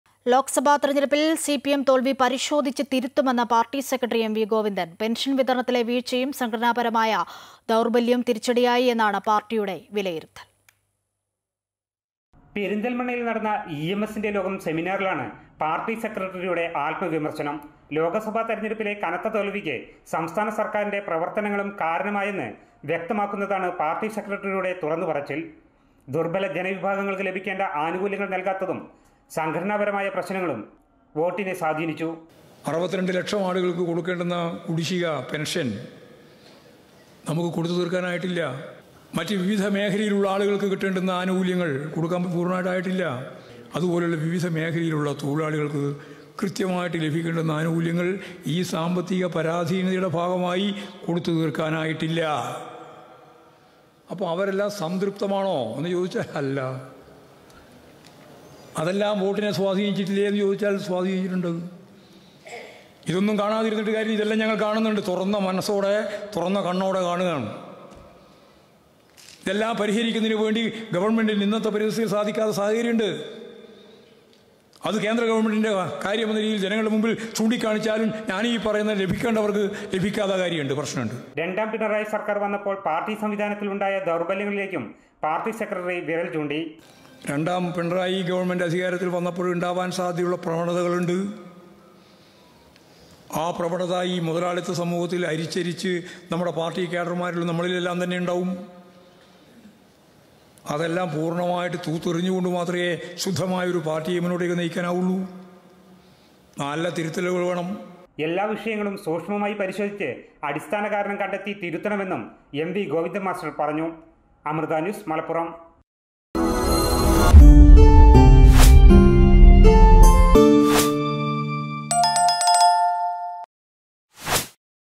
ലോക്സഭാ തെരഞ്ഞെടുപ്പ്: സിപിഎം തോല്‍വി പരിശോധിച്ച് തിരുത്തുമെന്ന് എം വി ഗോവിന്ദന്‍ | Amrita News
ലോകസഭ തിരഞ്ഞെടുപ്പിൽ സിപിഎം തോൽവി പരിശോധിച്ച് തിരുത്തുമെന്ന് പാർട്ടി സെക്രട്ടറി എം വി ഗോവിന്ദൻ.  പെൻഷൻ വിതരണത്തിലെ വീഴ്ചയും സംഘടനപരമായ ദൗർഭല്യവും തിരിച്ചടിയായി എന്നാണ് പാർട്ടിയുടെ വിലയിരുത്തൽ.